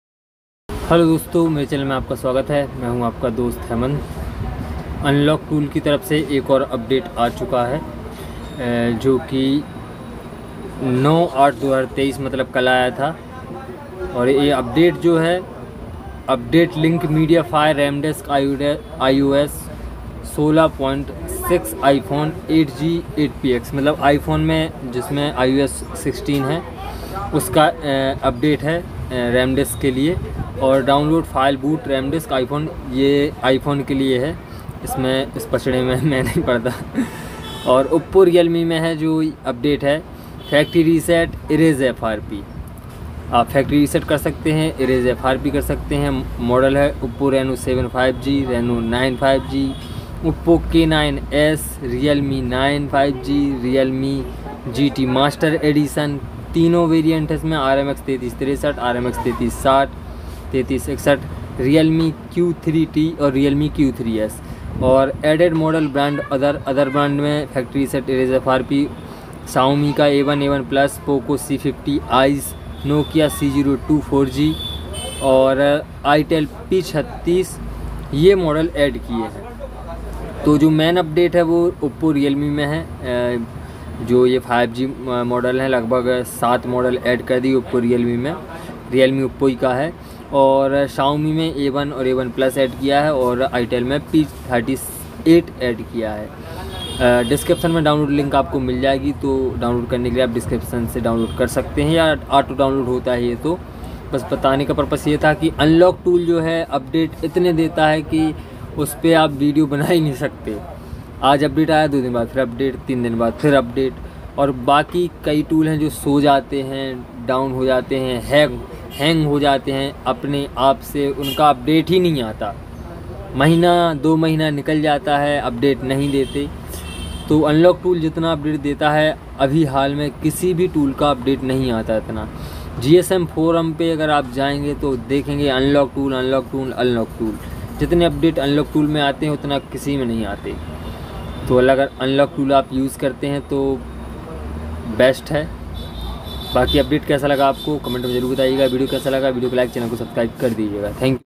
unlock tool new update v2023.08.09.0 || मोबाइल अनलोकिंग टूल तो एक ही है 😀😀
what's new:-

OPPO-REALME
- Factory Reset | Erase FRP
- Oppo Reno 7 5G PFJM10
- Oppo Reno 9 5G PHM110
- Oppo K9s PERM10
- Realme 9 5G Speed RMX3461
- Realme GT Master Edition RMX3363 | RMX3360 | RMX3361
- Realme Q3T RMX3462
- Realme Q3s RMX3461
Added Models Brand Other
- Xiaomi Redmi A1 | A1+ | POCO C50 ice
- Nokia C02 4G Cypher_00WW
- Symphony G26
- QSmart Rocket
- Mobicel VX18
- Itel P38
- ZTE Optus X Start 2